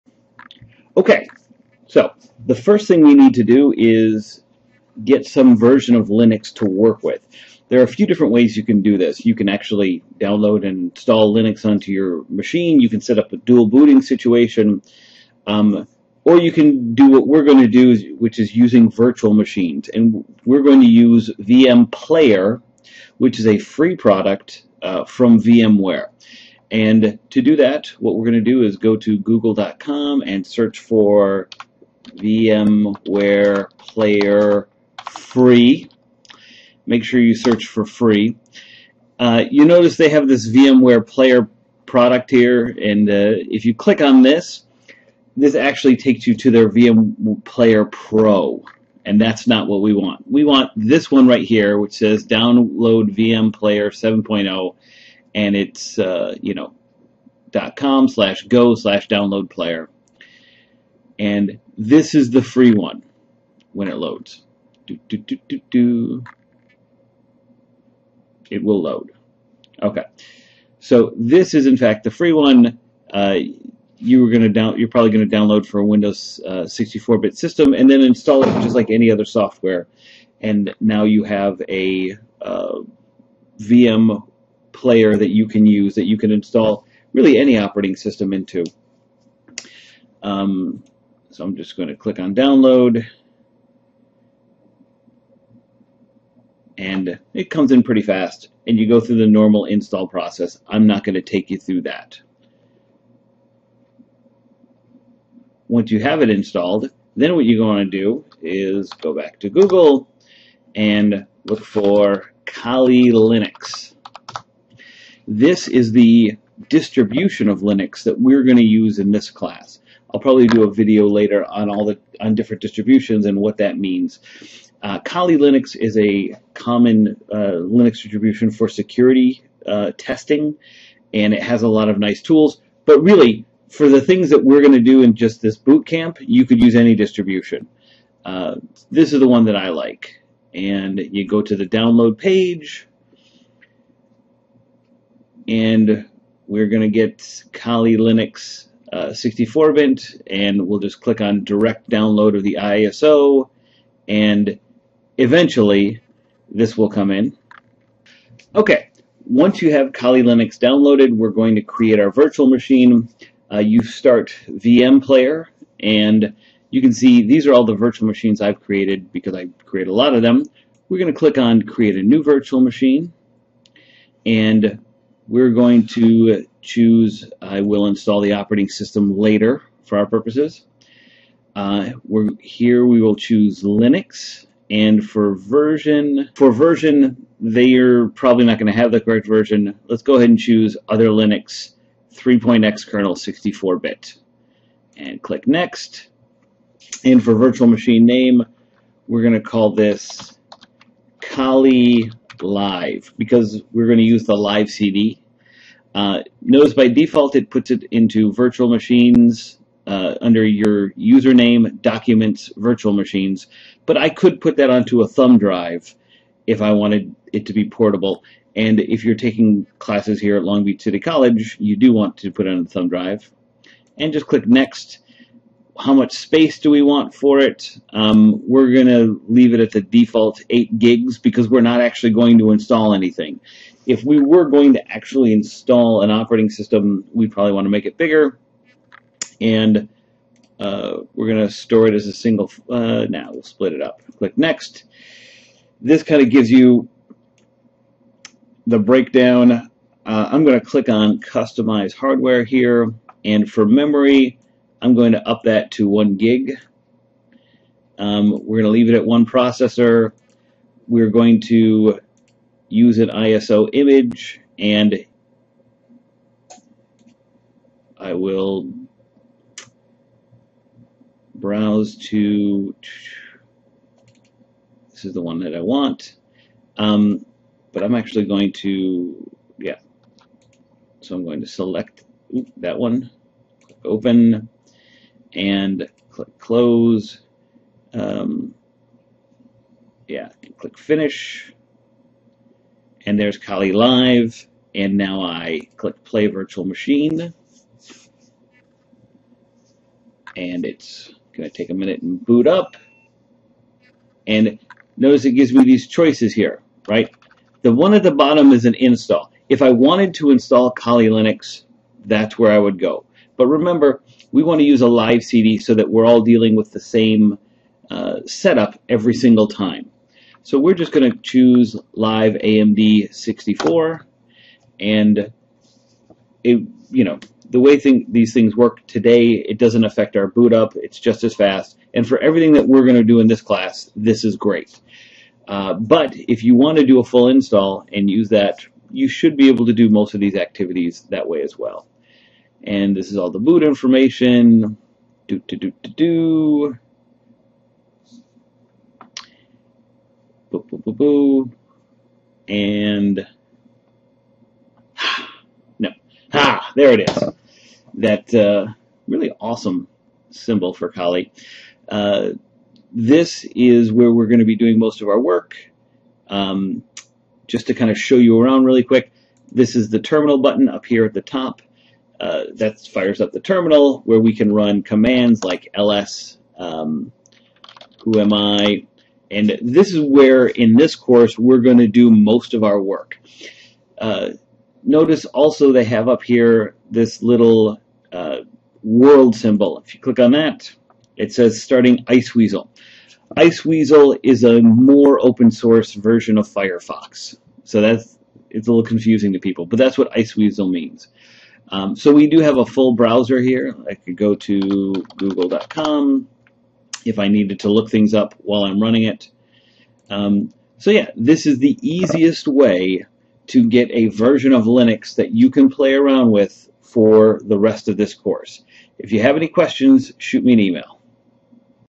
Big Bad Linux Bootcamp: Create Kali Live Virtual Machine
Big Bad Linux Bootcamp - How to create a VM using the Kali LiveCD for learning Linux